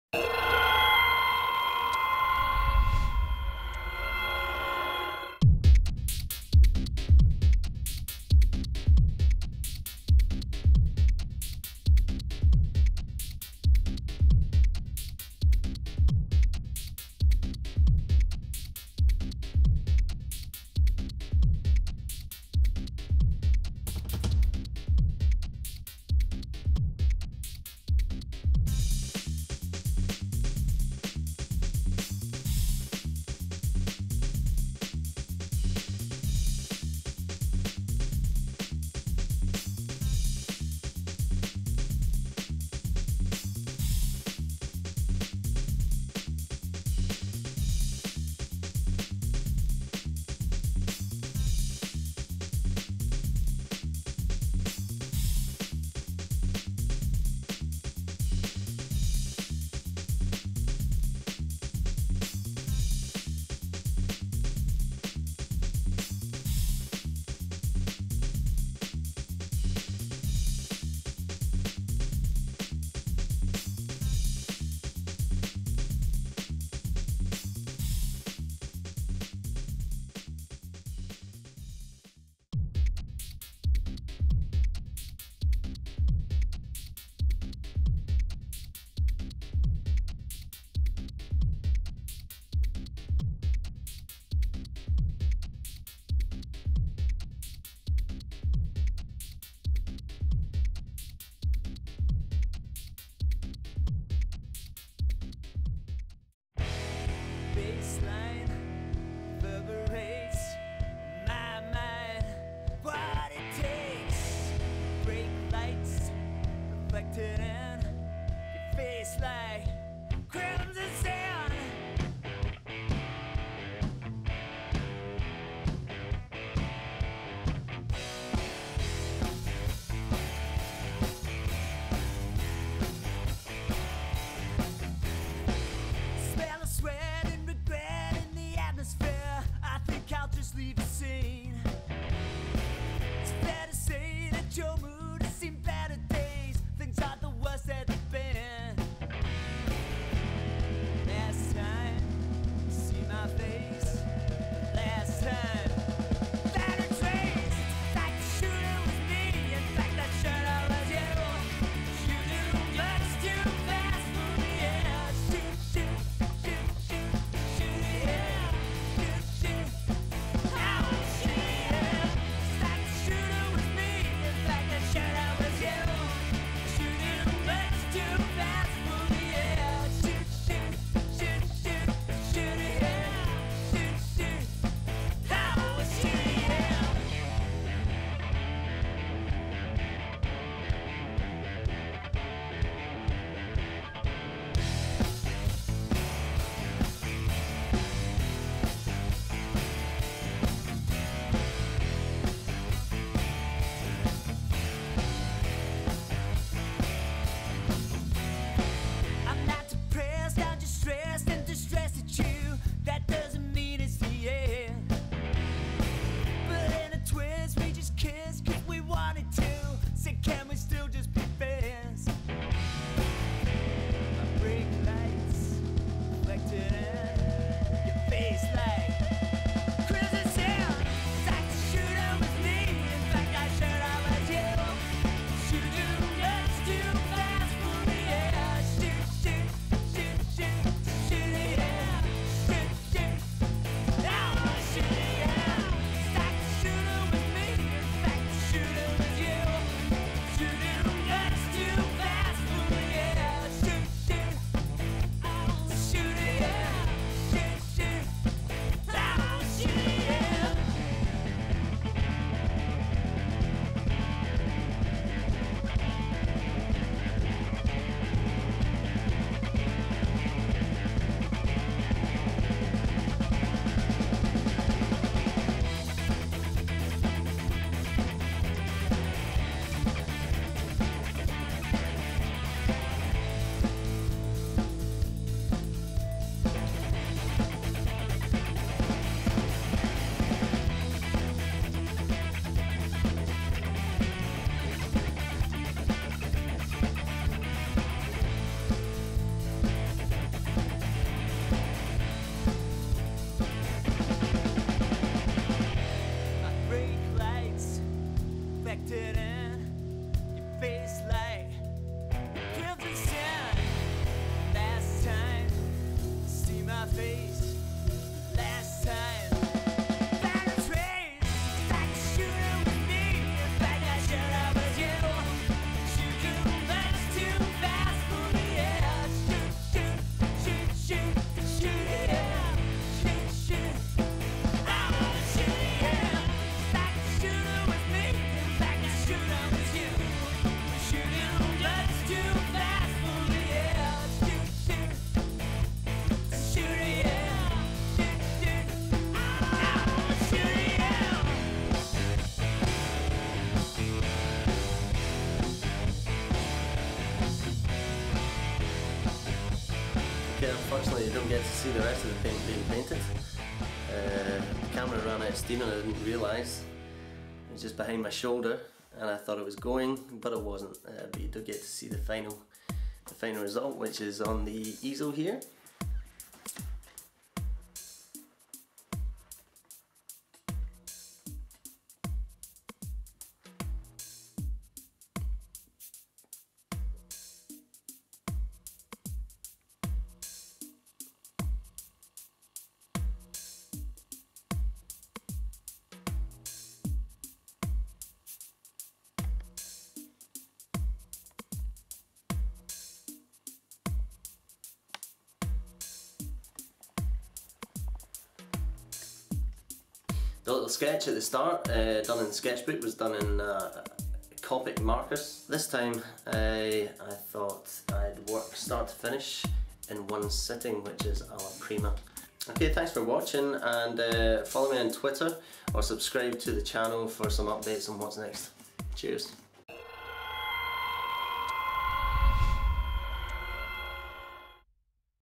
Sketch and Alla Prima Oil Painting: Old Man of Storr, Skye
Latest oil landscape painting from the studio: "Old Man of Storr".
The Storr is a rock formation on Trotternish, Skye just north of Portree.
This film shows how a sketch is prepared in Copic markers before the final painting is done in the studio in one sitting (Alla Prima).

About 2 thirds of the way through painting, my memory ran out in my video camera so unfortunately the final stages are not shown, but you do see the final painting.